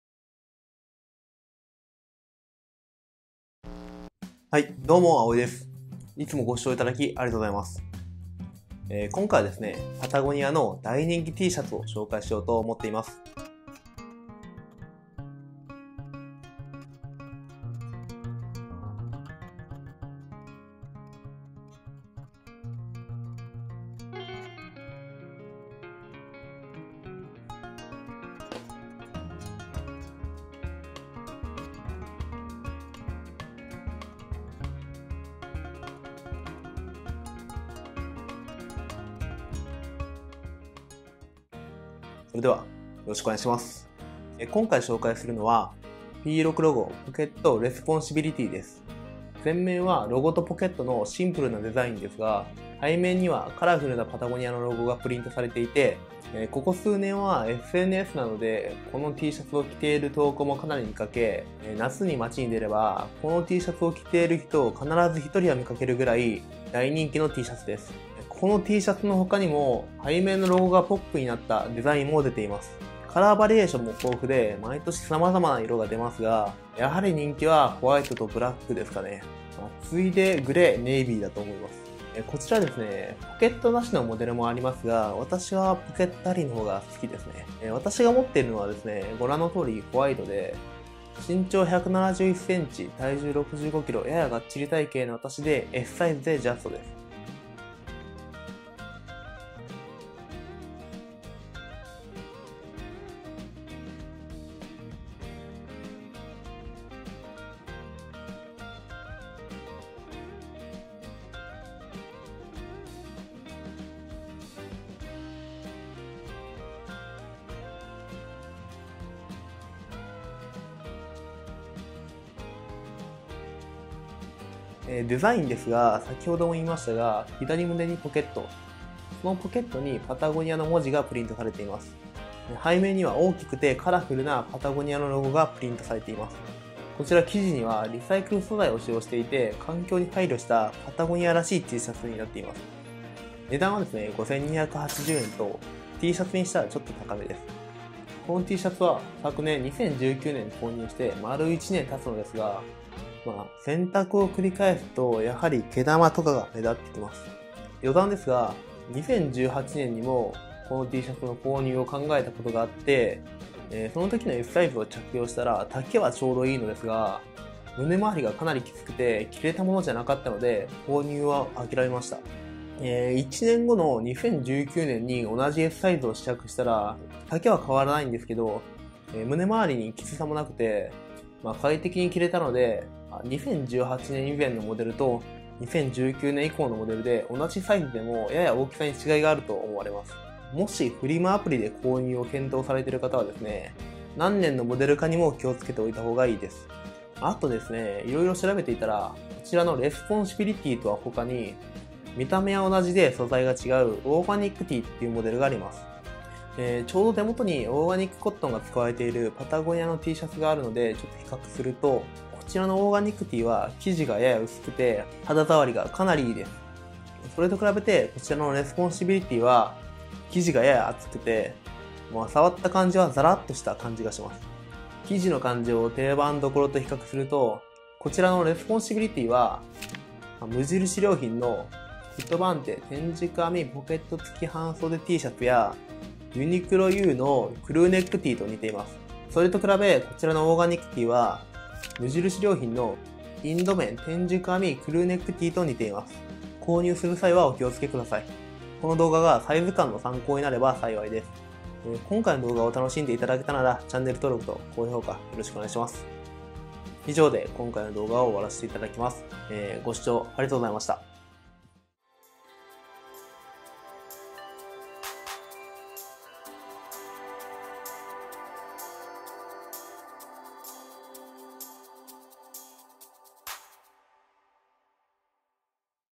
【patagonia】パタゴニアの大人気Tシャツ P-6ロゴ・ポケット・レスポンシビリティー
パタゴニアの大人気Tシャツ　P-6ロゴ・ポケット・レスポンシビリティーの紹介動画です。

【品名】
P-6ロゴ・ポケット・レスポンシビリティー

【カラー】
ホワイト

【値段】
￥5,280円

【サイズ】
Sサイズ

【私の体型】
１７１㎝６５㎏ややガッチリ体型

【ひとこと】
夏に街に出れば着ている人を必ず見かけるくらい大人気のTシャツです。

【目次】
00:00　はじめに
00:44　紹介１
01:37　XS・Sサイズ着用動画
02:05　紹介２
02:28　購入の際の注意点
03:17　オーガニックティーとの比較
04:09　おわりに